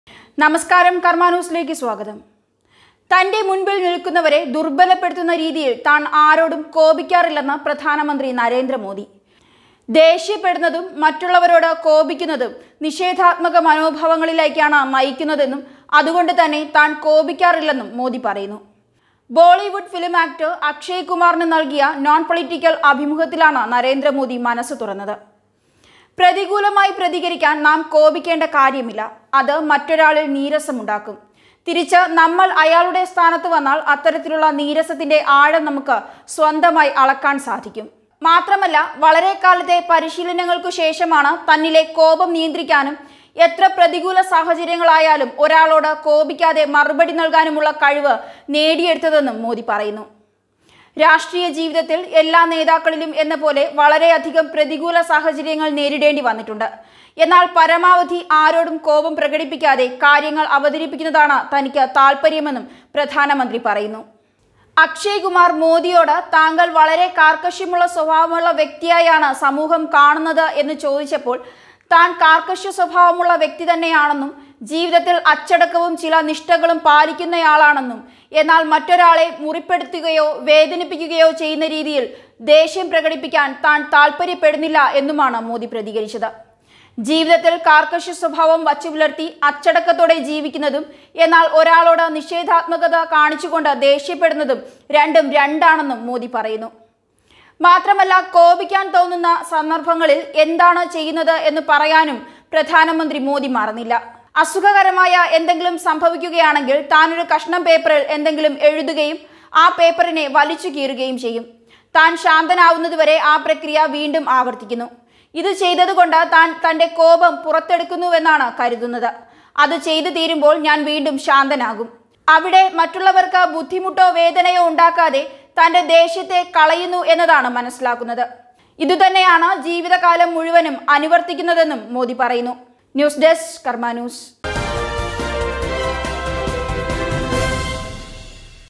ഇതാണ് പച്ചയായ നമ്മുടെ പ്രധാനമന്ത്രി ശത്രുക്കൾ പോലും ബഹുമാനിക്കും ഈ വ്യക്തിത്ത്വത്തെ_karmanews
അസുഖകരമായ എന്തെങ്കിലും സംഭവിക്കുകയാണെങ്കിൽ, ഞാനൊരു കഷണംപേപ്പറിൽ എന്തെങ്കിൽം എഴുതുകയും ആ പേപ്പറിനെ വലിച്ചു കീറുകയും ചെയ്യും. ഞാൻ ശാന്തനാവുന്നതുവരെ ആ പ്രക്രിയ വീണ്ടും